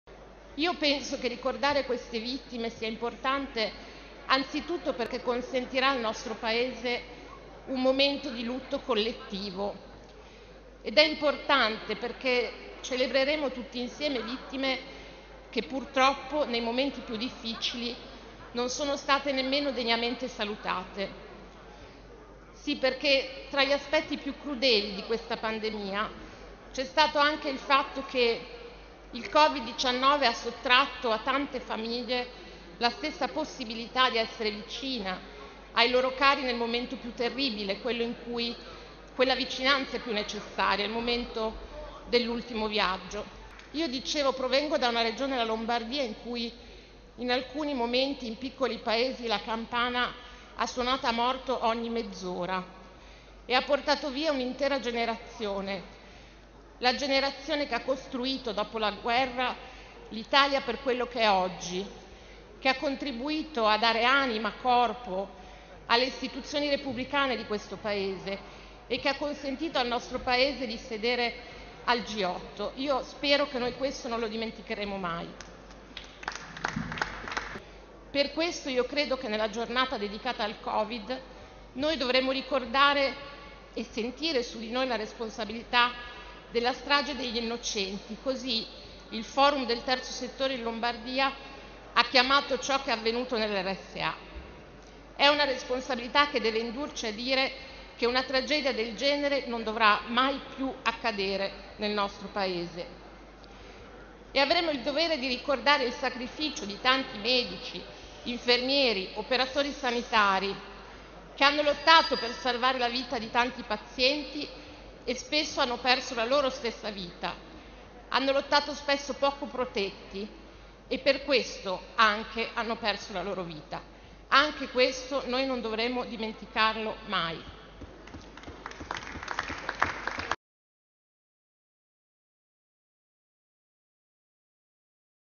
MEMORIA, RISPETTO E PREVENZIONE. LI DOBBIAMO ALLE VITTIME DEL COVID-19
Ricordare le vittime del covid-19 consentirà al nostro Paese un momento di lutto collettivo, ci permetterà di onorare quelle vittime della generazione che ha ricostruito l'Italia e l'ha fatta grande, ci permetterà di riflettere su quanto successo nelle RSA e di ricordare il sacrificio di tanti medici, infermieri, operatori sanitari che hanno tanto duramente lottato contro il virus rimettendoci, purtroppo, la vita.

Lisa Noja